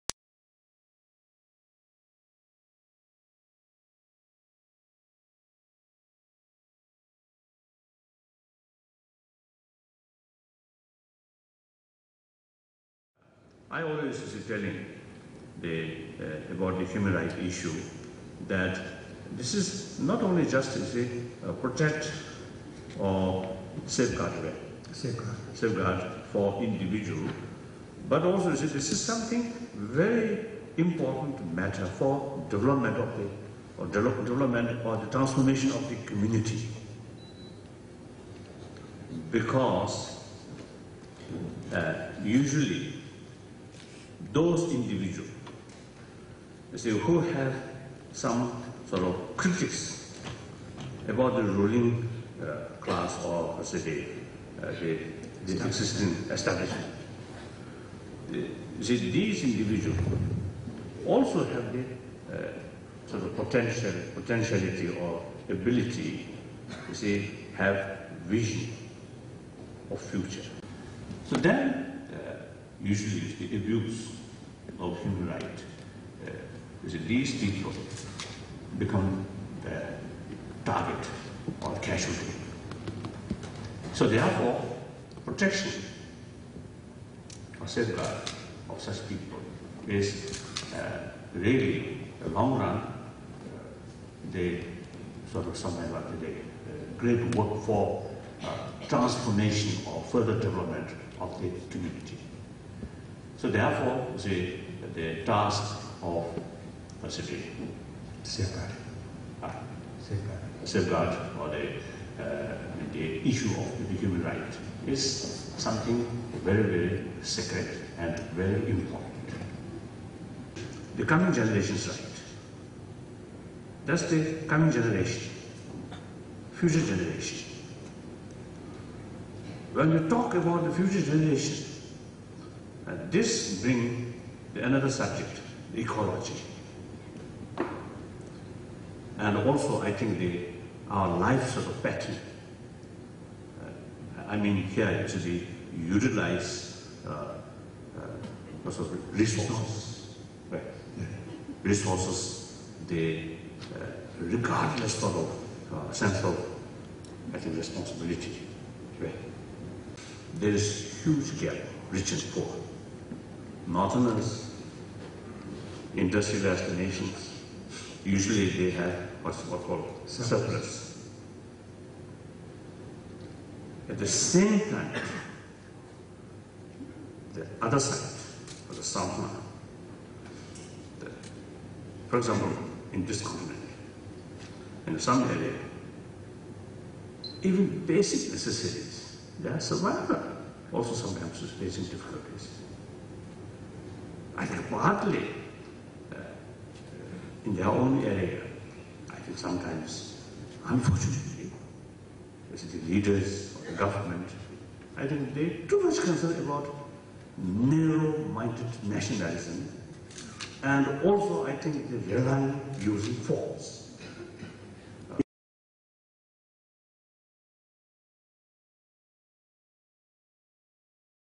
Dalai Lama's Speech in Durban Part 2
His Holiness Dalai Lama's speech at the Universal Declaration of Human Rights International Print Portfolio Opening at the Durban Art Gallery (10 December 1999). Part 2